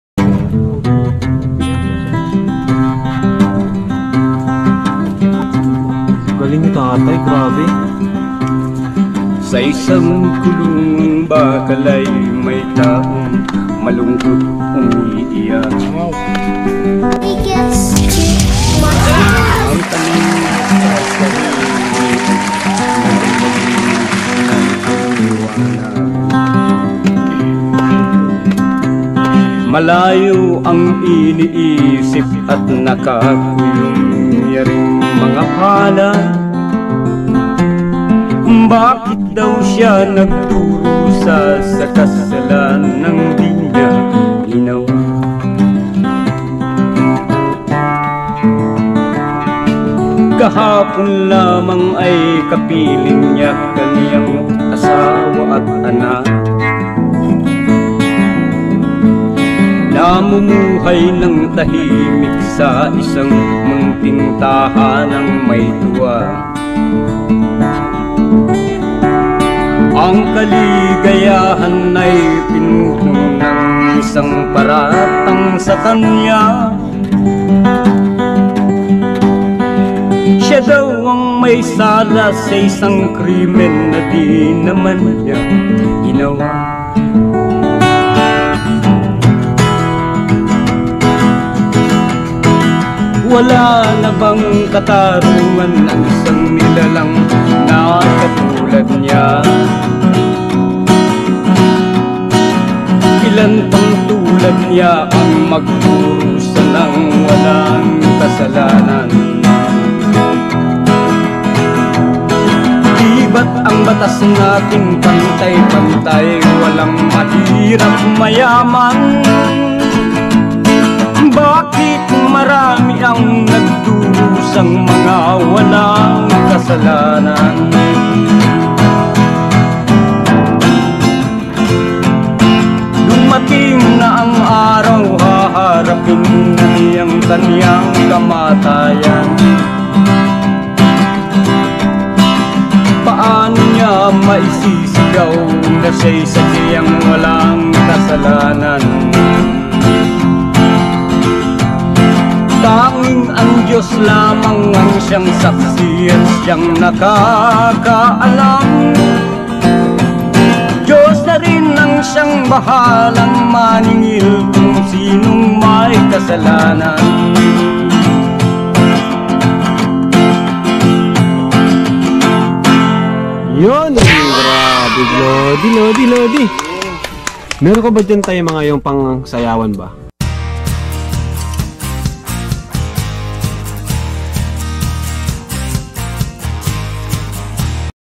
Katarungan by kuya Randy@gintongguntingvlog5169
Ang video vlog na ito ay pag-aari ni kalingaprab valsantosmatubang BenteBoy.Ang gustong magbigay Ng tulong kay Kuya Randy. puntahan nyo nalang pi yung Facebook account ni#kalingaprab or#valsantosmatubang